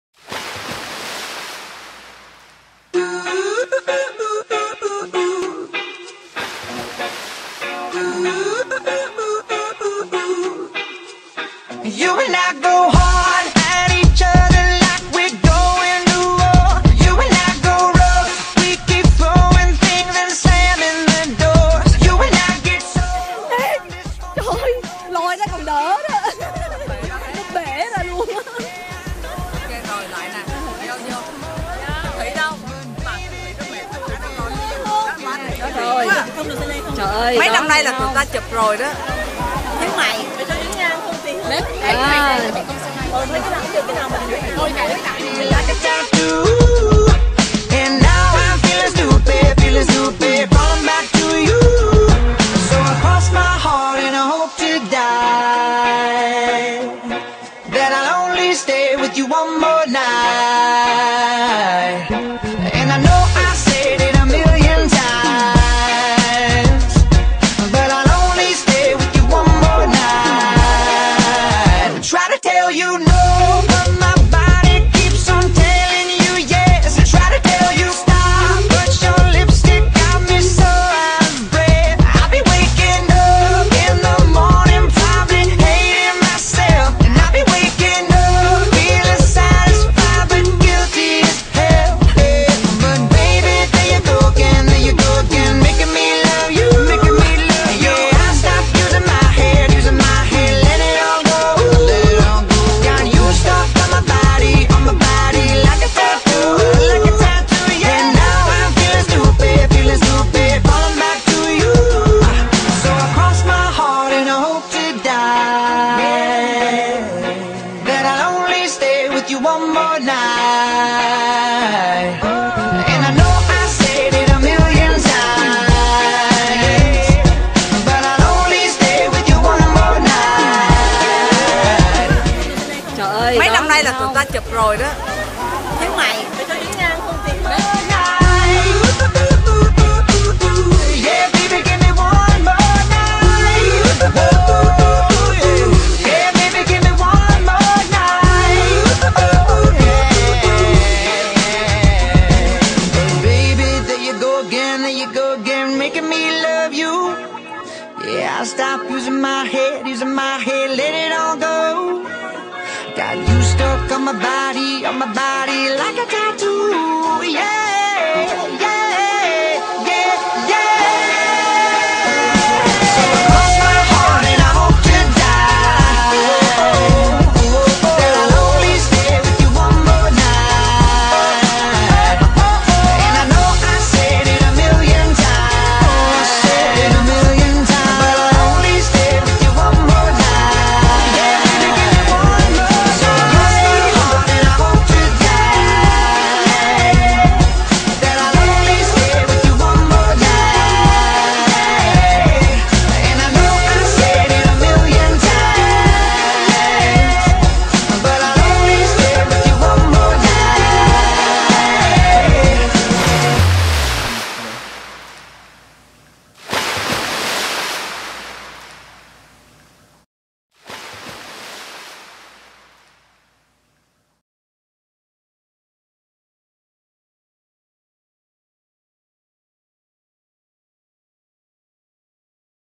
40 Nam Tinh Ban/ Gia Long 72-79/ Lop 6/6
Mot chut gi de nho va de thuong cua thua hoc tro xa xua voi 40 Nam Tinh Ban. I LOVE YOU, cac ban Gia Long. HTThuy